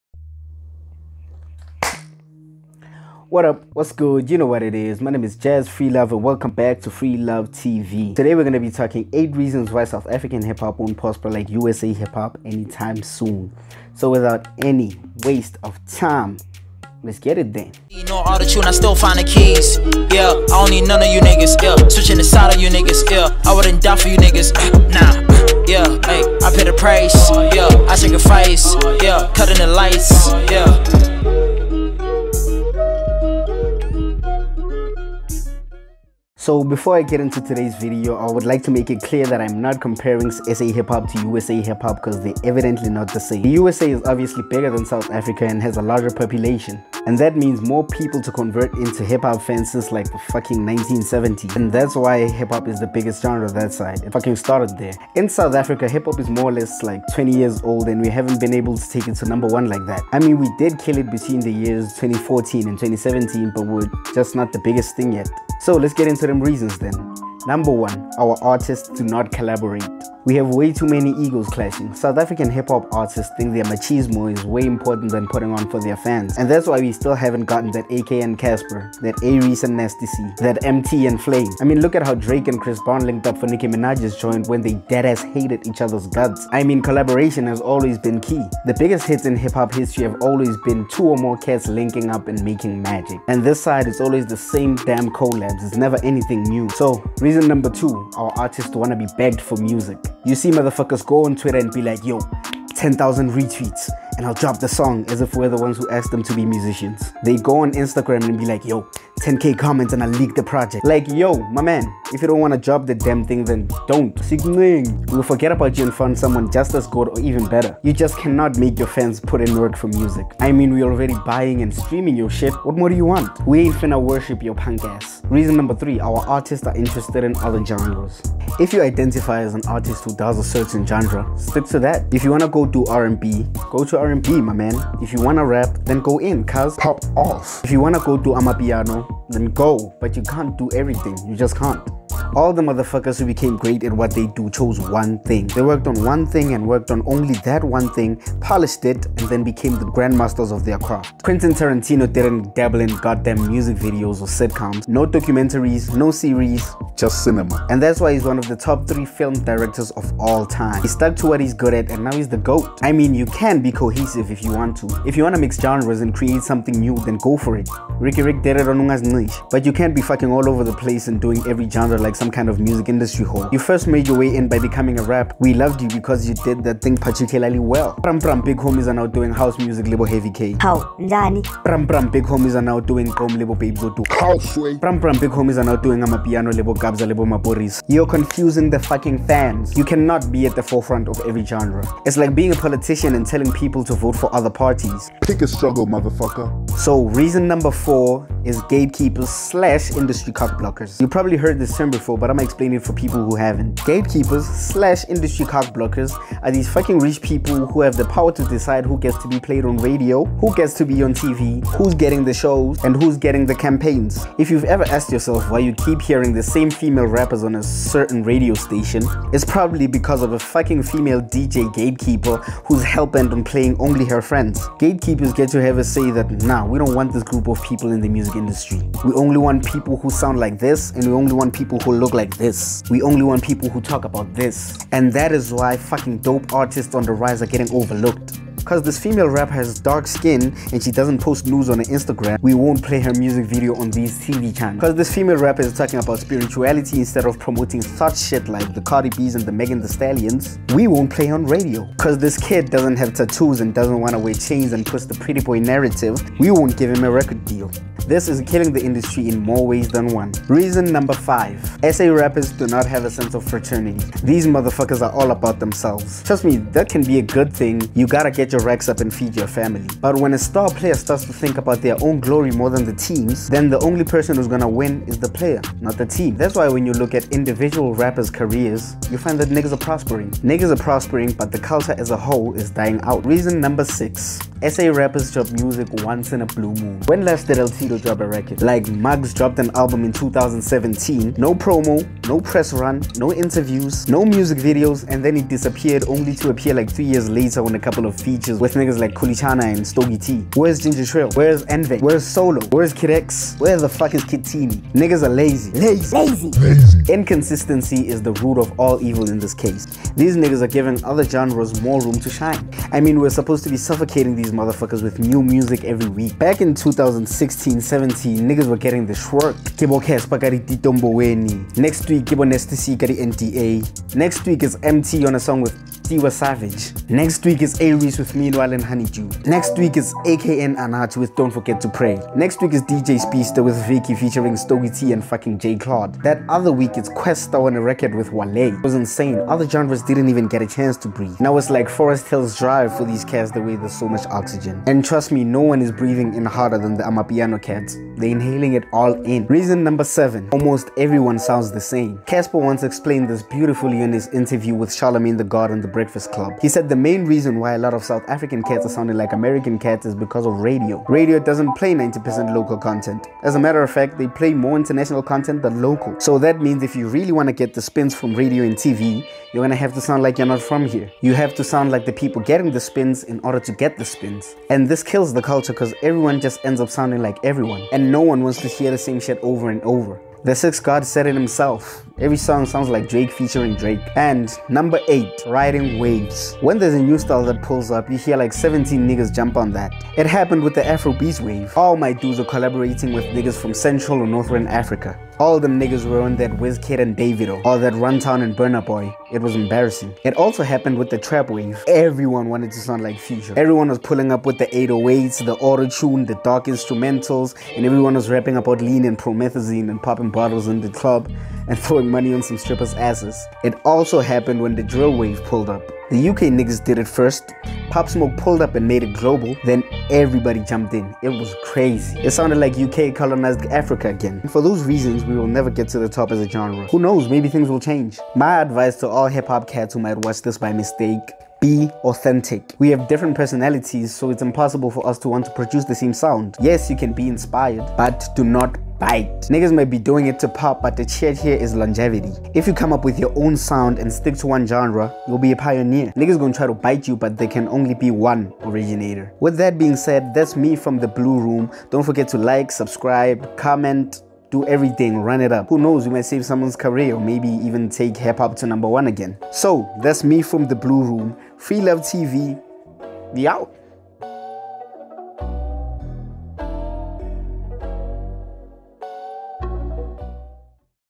8 Reasons why SA Hip-Hop Is not #1 w/Jazze
Watch as Jazze breaks down an 8 reason list on why SA Hip-Hop isn't hitting as hard as it should.

I also made a video on why Uncle Vinny is getting so much social media hate